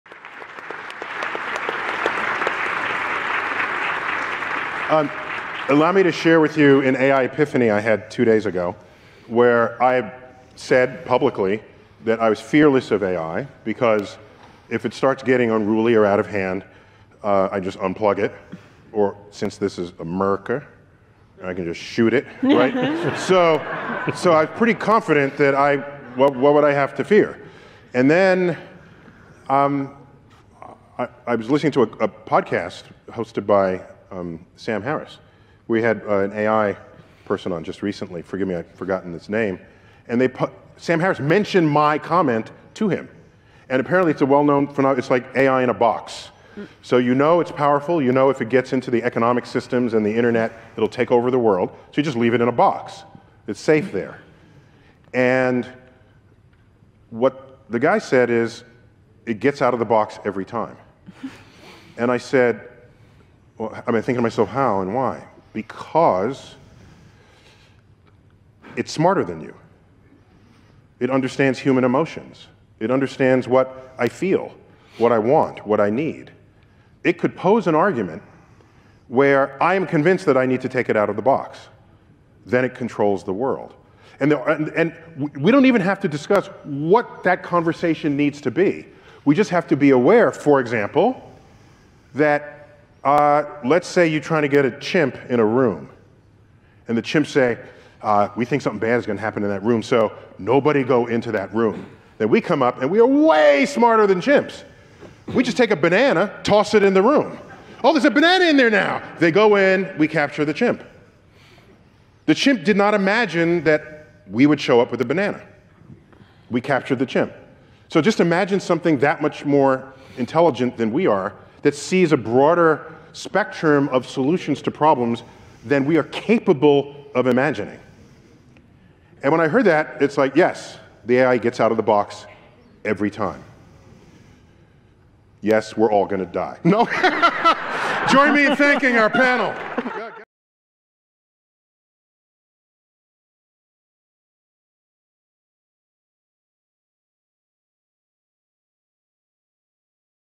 Neil deGrasse Tyson Now Fears A.I.
Neil deGrasse Tyson details his epiphany on AI from listening to Sam Harris' podcast while moderating the 2018 Isaac Asimov Memorial Debate on artificial intelligence: "It gets out of the box, every time."

Panel:
John Giannandrea (Senior Vice President of Engineering, Google),
Helen Greiner (Co-Founder, iRobot; Founder, CyPhy Works)
Ruchir Puri (Chief Architect, IBM Watson)
Max Tegmark (Professor of Physics, Massachusetts Institute of Technology; President, Future of Life Institute)
Michael P. Wellman (Professor of Computer Science and Engineering, University of Michigan)